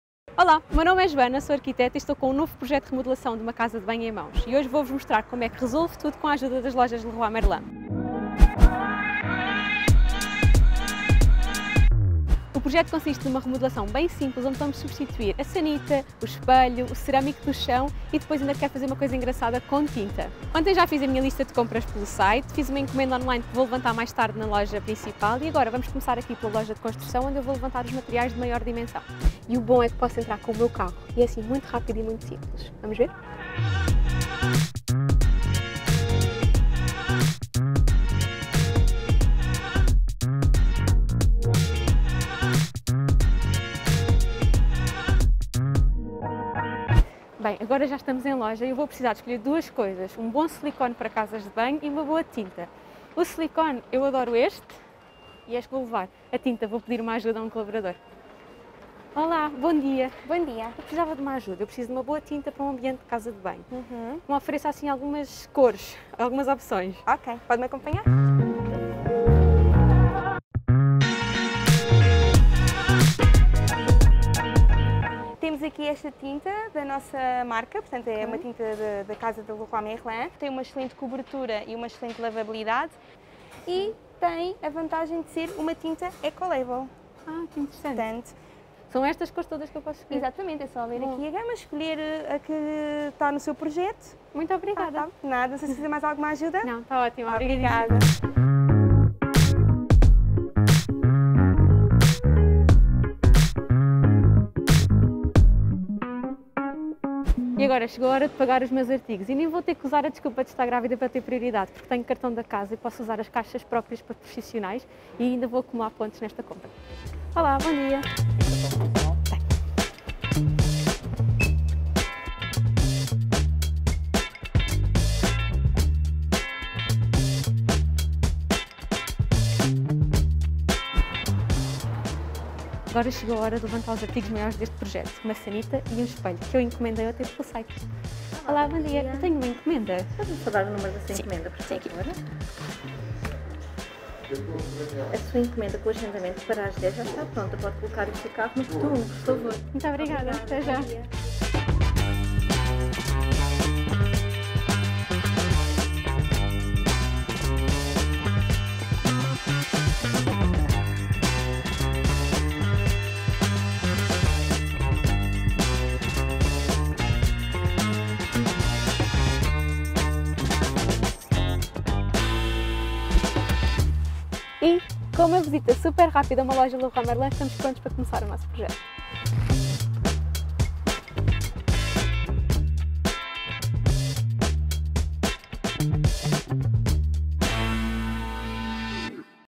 Elaboração de um projeto com a LEROY MERLIN, com Joana Laranjeira
A arquiteta Joana Laranjeira, do @oda_joana, tem um novo projeto de remodelação de uma casa de banho e vai mostrar-nos como é que, com a ajuda de uma loja LEROY MERLIN, prepara a elaboração do projeto.